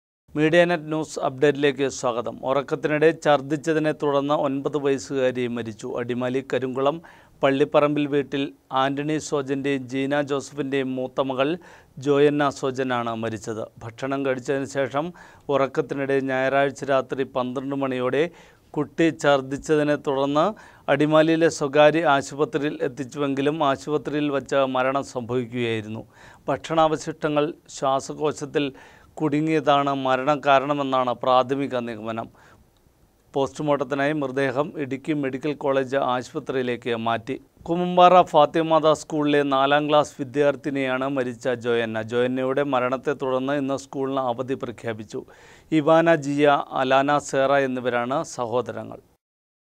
ഒന്‍പത് വയസുകാരിക്ക്‌ ദാരുണാന്ത്യം
ഉറക്കത്തില്‍ ഛര്‍ദ്ദിച്ചതിനെത്തുടര്‍ന്ന് ഒന്‍പത് വയസുകാരി മരിച്ചു.അടിമാലികരിങ്കുളം പള്ളിപ്പറമ്പില്‍ വീട്ടില്‍ ആന്റണി സോജന്റെയും ജീനാ ജോസഫിന്റെയും മൂത്തമകള്‍ ജോയന്ന സോജനാണ് മരിച്ചത്. ഭക്ഷണം കഴിച്ചതിന് ശേഷം ഉറക്കത്തിനിടെ ഞായറാഴ്ച രാത്രി 12 മണിയോടെ  കുട്ടി ഛര്‍ദിച്ചതിനെത്തുടര്‍ന്ന് അടിമാലിയിലെ സ്വകാര്യ ആശുപത്രിയില്‍ എത്തിച്ചുവെങ്കിലും ആശുപത്രിയില്‍ വച്ച് മരണം സംഭവിക്കുകയായിരുന്നു. ഭക്ഷണാവശിഷ്ടങ്ങങ്ങള്‍ ശ്വാസകോശത്തില്‍ കുടുങ്ങിയതാണ് മരണകാരണമെന്നാണ് പ്രാഥമിക നിഗമനം.പോസ്റ്റുമോര്‍ട്ടത്തിനായി മൃതദേഹം ഇടുക്കി മെഡിക്കല്‍ കോളേജ് ആശുപത്രിയിലേക്ക് മാറ്റി.സംസ്‌കാരംമൂന്നാര്‍ മൗണ്ട് കാര്‍മ്മല്‍ ബസലിക്കയില്‍ വൈകുന്നേരം 4 മണിയ്ക്ക് നടക്കും. കൂമ്പന്‍പാറ ഫാത്തിമ മാത സ്‌കൂളിലെ നാലാം ക്ലാസ് വിദ്യാര്‍ത്ഥിനിയാണ് മരിച്ച ജോയന്ന. ജോയന്നയുടെ മരണത്തെത്തുടര്‍ന്ന് ഇന്ന് സ്‌കൂളിന് അവധി പ്രഖ്യാപിച്ചു.ഇവാന  ജിയ, അലാന സേറ എന്നിവരാണ് സഹോദരങ്ങള്‍.